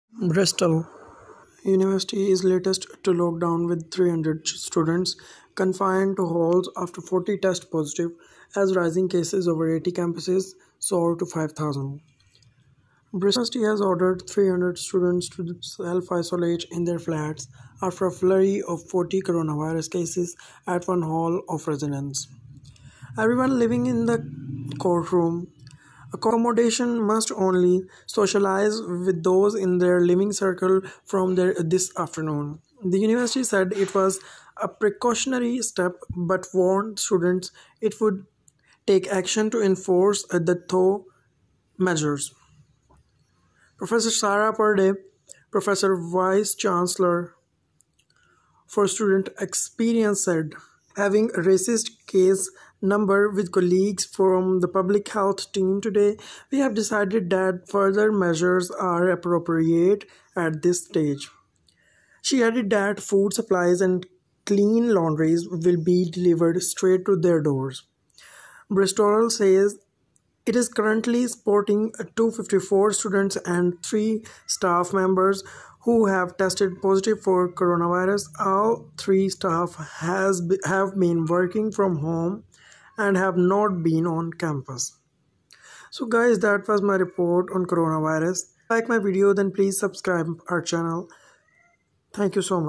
Bristol University Covid-19 arise.. || Breaking News.. || Tech News..
Bristol University is latest to lock down with 300 students confined to halls after 40 test positive - as rising cases over 80 campuses soar to 5,000
Bristol University has ordered 300 students to self-isolate in their flats after a flurry of 40 coronavirus cases at one halls of residence.

Everyone living in The Courtrooms accommodation must 'only socialise with those in their living circle from this afternoon'.
The University said it was a precautionary step but warned students it would 'take action' to enforce the tough measures.

Professor Sarah Purdy, Pro Vice-Chancellor for Student Experience, said: 'Having reassessed case numbers with colleagues from the Public Health Team today, we have decided that further measures are appropriate at this stage.

She added that food, supplies and clean laundry will be delivered straight to their door.

Bristol says it is currently supporting 254 students and three staff members who have tested positive for coronavirus. All three staff have been working from home and have not been on campus.

By i_siki_writes ...

◼️ Voice is performed by our Voice Over Artist.
◼️ We offer you good quality content on YouTube.